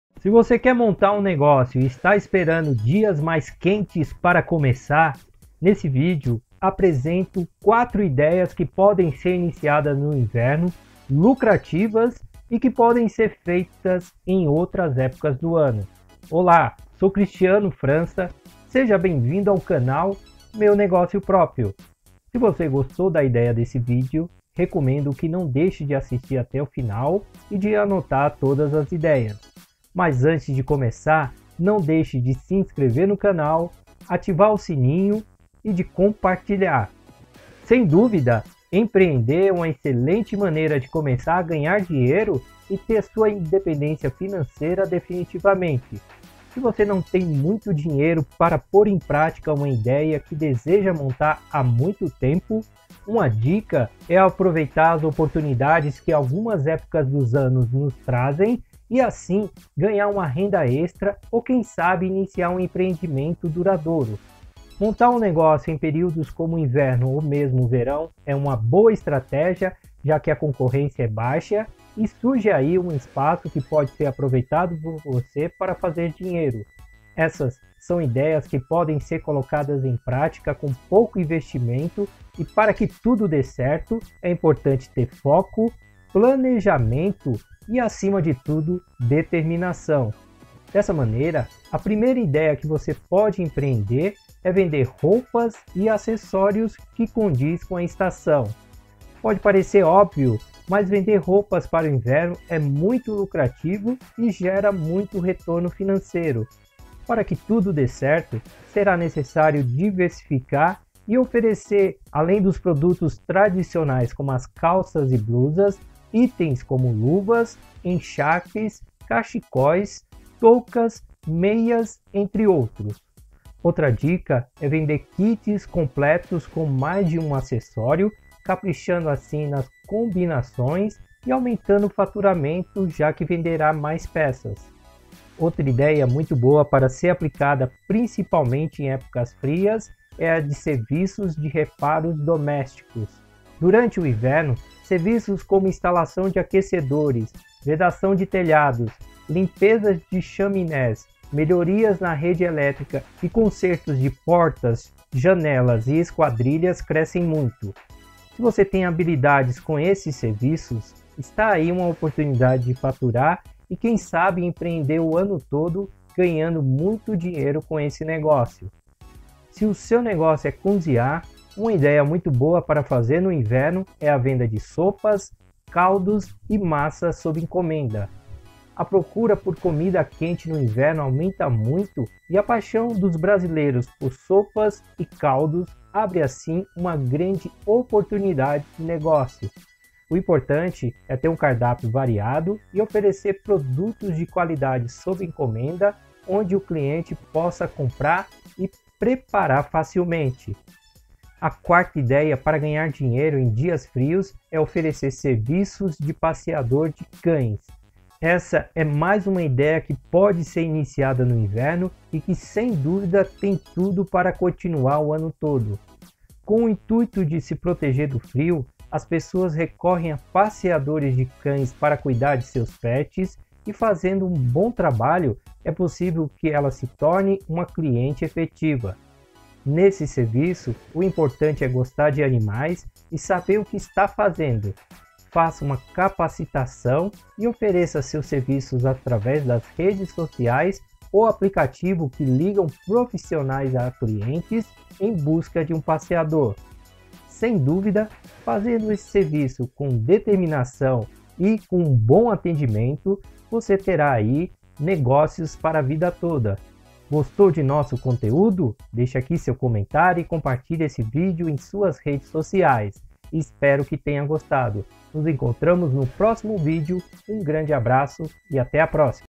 O que Vender [FAZER] no Inverno Para Ganhar Dinheiro
O que Vender no Inverno Para Ganhar Dinheiro
👉 Conheça o BLOG MEU NEGÓCIO PRÓPRIO

O que Vender ou Fazer no Inverno Para Ganhar dinheiro

Se você está atrás de ideias de negócios para Ganhar Dinheiro no Inverno ou mesmo em outras épocas do ano, nesse vídeo indicamos ideias de empreendimentos que tem grande procura no inverno e que são uma porta de entrada para quem deseja empreender.